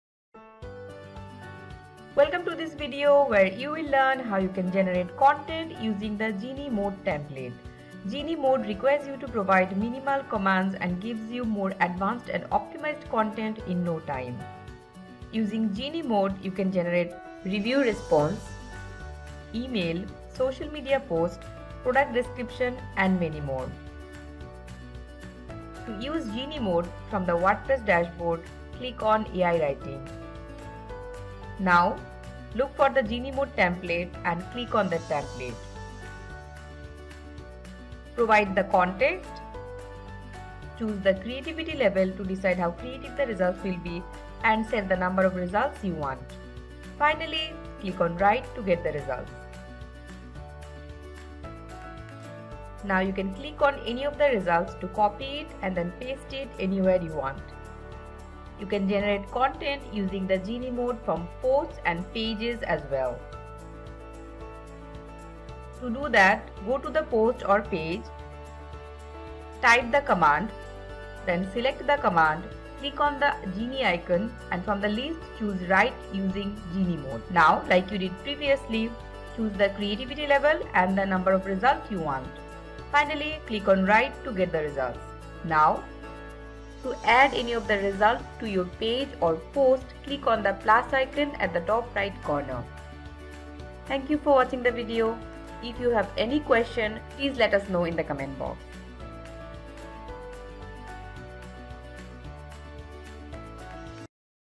How to Generate Any Content with Genie Mode || GetGenie || Conversational Generative AI
In this video, we have shown how you can use Genie Mode template of GetGenie. This is an advanced feature that empowers you to generate various types of content with minimal command . With Genie Mode, you can generate :

➡️ Product Review
➡️ Product Description
➡️ Application  and many more.

🔔 You can check out the process of  GetGenie installation and claiming free licne in this video